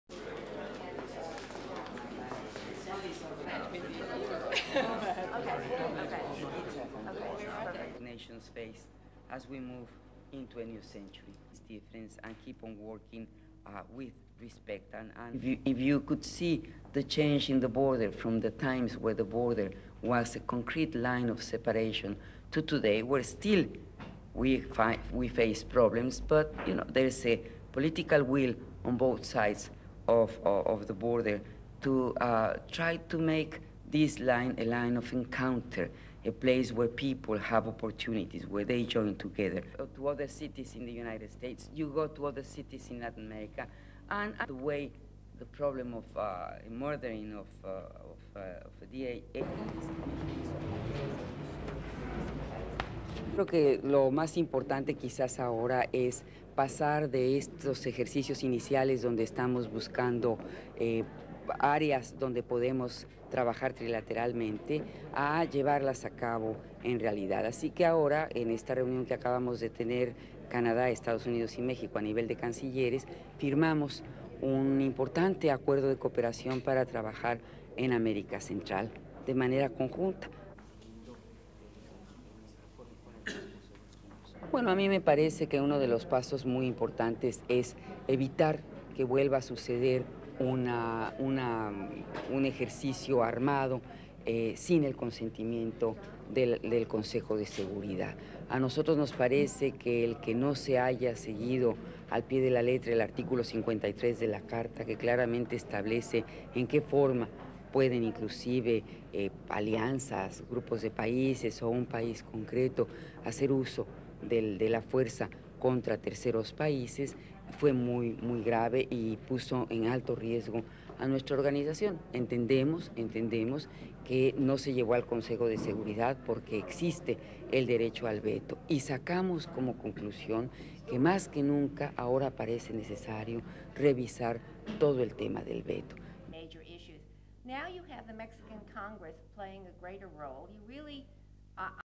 USA: MEXICAN FOREIGN MINISTER GREEN TALKS ON DRUGS TRADE
(27 Sep 1999) Eng/Spanish/Nat

Mexican Foreign Minister Rosario Green was in New York on Monday to talk about her country's biggest issues with its neighbours.

She commented on border issues and the drug trade, topics which have long been at the forefront of Mexican-American relations.

But she also talked about the recent efforts to communicate and cooperate on a myriad of subjects.

Saying that sometimes nations must agree to disagree, Mexican Foreign Minister Rosario Green, outlined ways Canada, the United States and Mexico could work together to find mutual solutions to common problems.

She said delegations from all three countries had consulted each other to work out answers to drug and border problems.

SOUNDBITE: (English)
"If you could see the change in the border from the time when the border was a complete line of separation till today, we still face problems but there is a political will on both sides of the border to try to make this line a line of encounter, a place where people have opportunity, where they join together."
SUPER CAPTION: Rosario Green, Secretary of Foreign Affairs, Mexico

SOUNDBITE: (Spanish)
"I think perhaps the most important thing right now is to pass these initial meetings where we are looking for areas where we can work together trilaterally and where we can succeed in them.  Right now Canada the U.S. and Mexico are having meetings with the ministers. We signed an important agreement of cooperation to work together in Central America as a joint effort."

SOUNDBITE: (Spanish)
"Well, I think for me one of the most important steps is to avoid the reoccurrence of a military manoeuvre, invading, without the consent of the Security Council. It seems like the letter of law was not followed in Article 53 which clearly establishes in which way alliances, groups of countries, one specific country can use force against against a third country.  It was very serious and put our organization at great risk. We understand that it didn't get approval of the Security Council because of the existence of the veto and we draw the conclusion that more than ever it seems necessary to revise the whole concept of the veto."